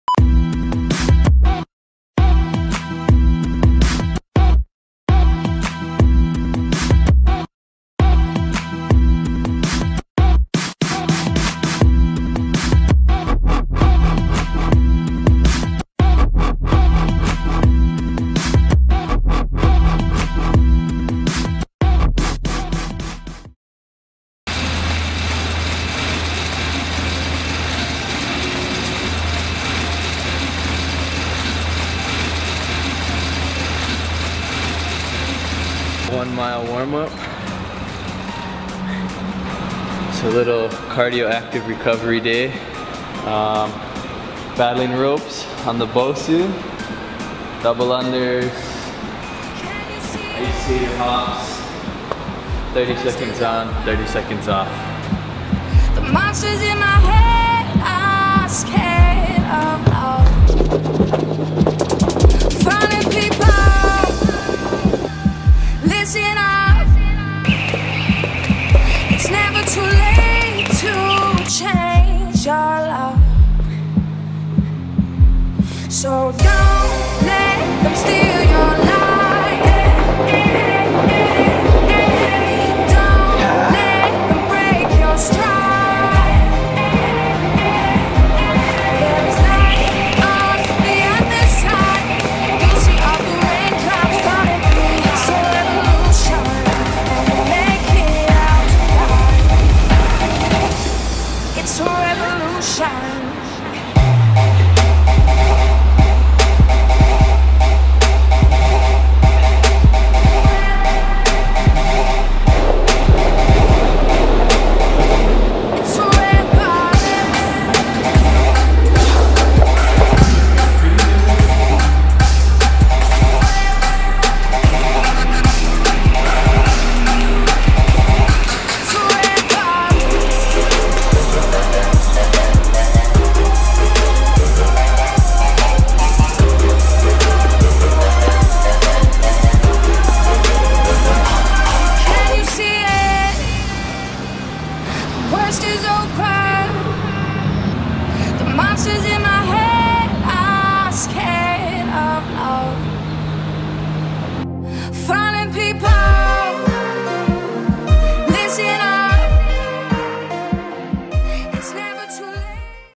HIIT Circuit (The best surf cardio)
6 Rounds. 30 seconds on, 30 seconds rest. This is great cardio for surfers and anyone else looking to sweat.